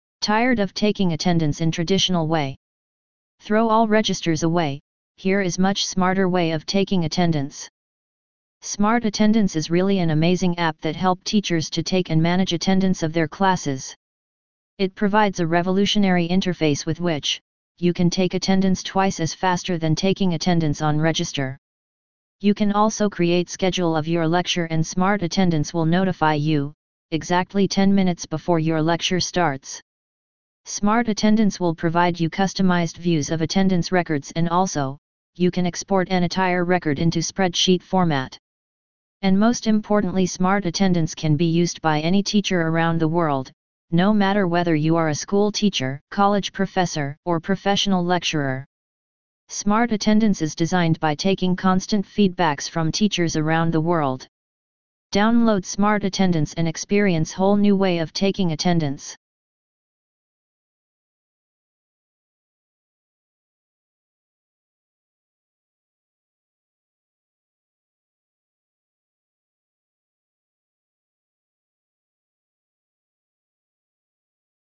Smart Attendance Android App
Smart Attendance is an amazing app for teachers that helps to take and manage attendance of their classes.
This app can be used by any teacher on this planet, no matter whether you are a primary school teacher, college professor or professional lecturer.

Smart Attendance provides customized views to see attendance records of student and further this records can be exported in spreadsheet format.

Now you don't need to remember the time of your next lecture, just add your lecture details and schedule it. Smart Attendance will automatically remind you 10 minutes before your lecture starts.

Further you can view your weekly schedule details.

Download Smart Attendance and be a digital teacher of digital age.